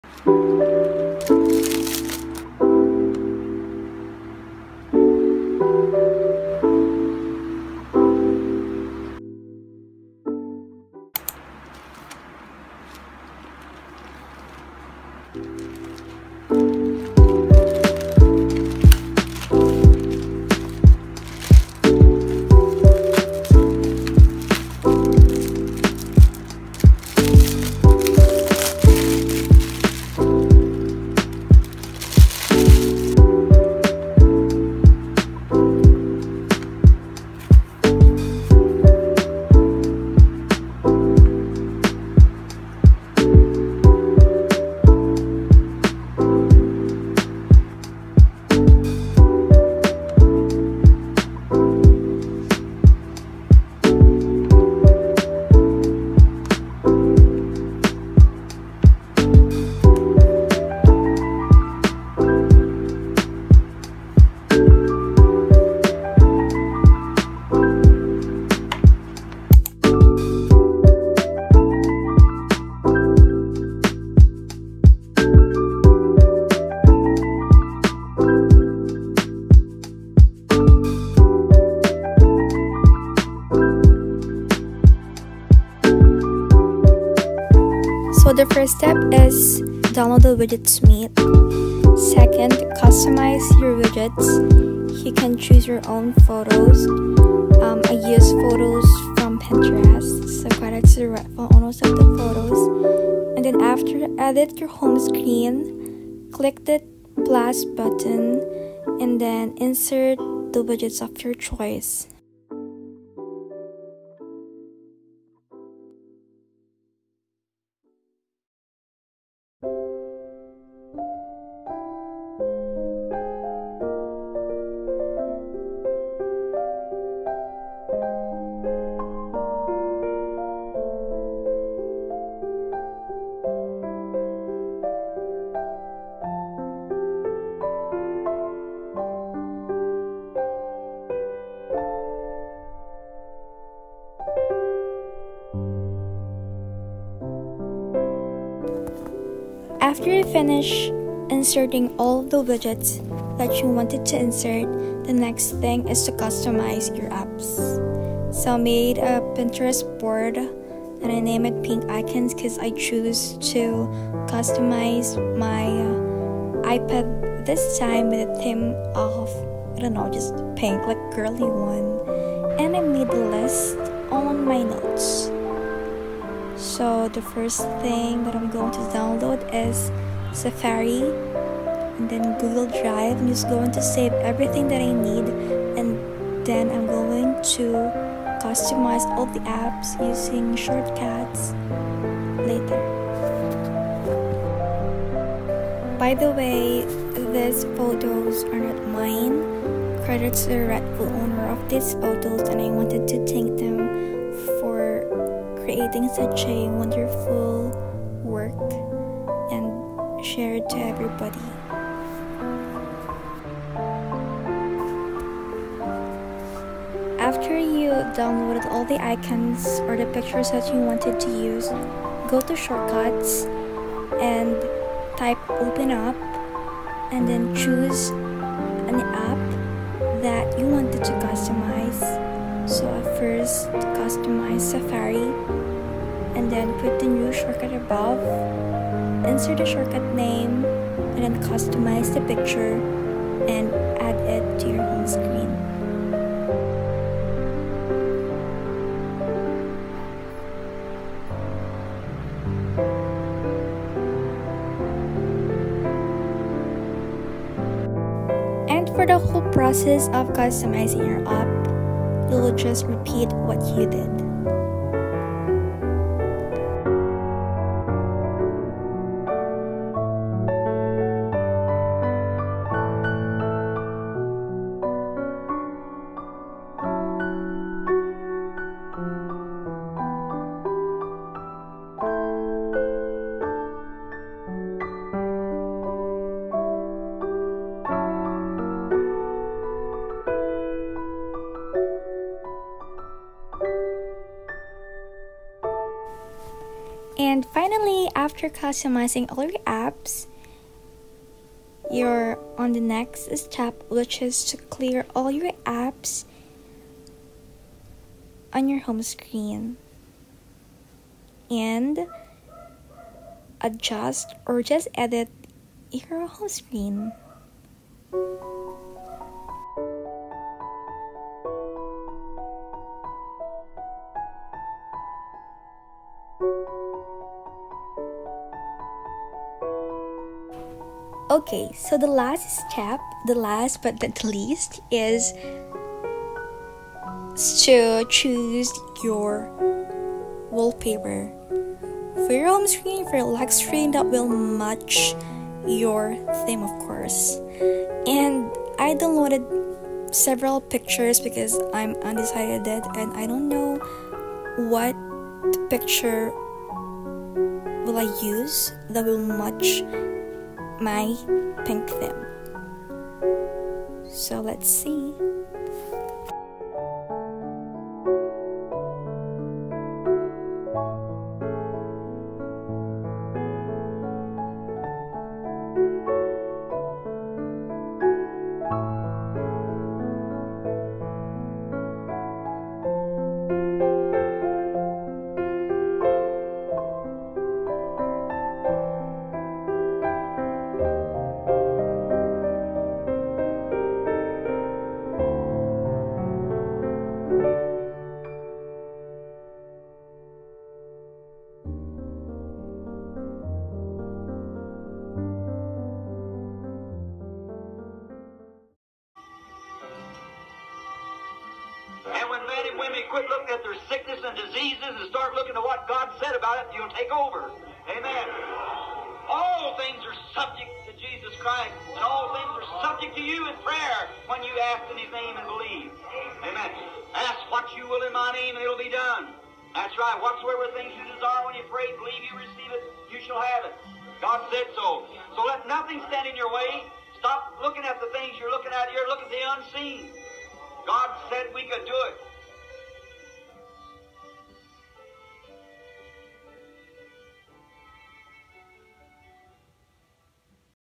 🌱Ipad transformation (ios14 customization ) | unboxing accessories
Hello, friends! Welcome or welcome back! How you doing? This is Mary and I do things I love! I finally had the chance to transform my iPad Air after a year😂 and to have this small giveaway! Hope everyone of you will join and I’m looking forward to see you on my next giveaways and videos in the future!

Always wear your mask and be safe, besties! God bless you!

Love, Mary.